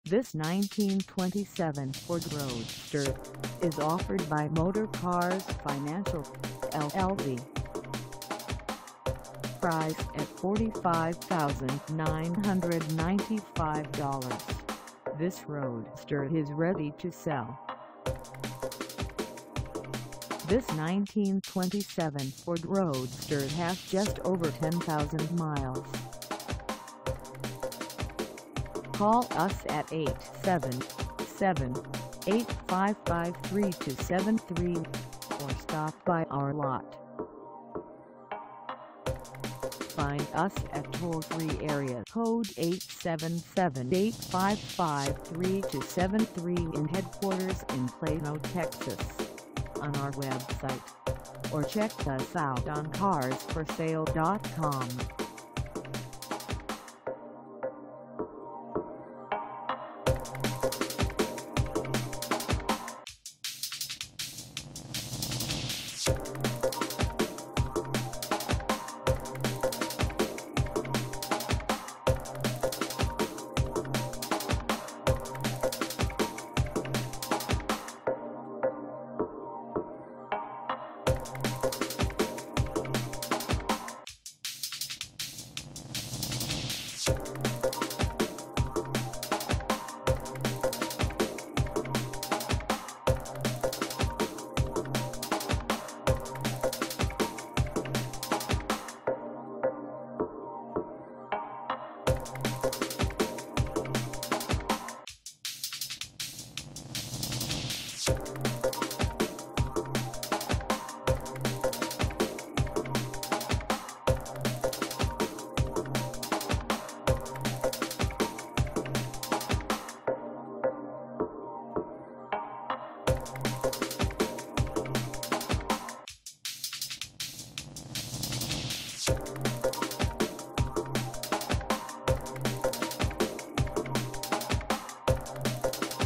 1927 Ford Roadster For Sale Convertible for sale in Headquar
This 1927 Ford Roadster For Sale Convertible is for sale in Headquarters in Plano, TX 75086 at Motorcars Financial, LLC.
Engine: Unspecified 350
Transmission: Unspecified
Color: Tangelo pearl
Mileage: 10000
Contact Motorcars Financial, LLC Toll Free Headquarters in Plano, TX 75086 to test drive this 1927 Ford Roadster For Sale Convertible today.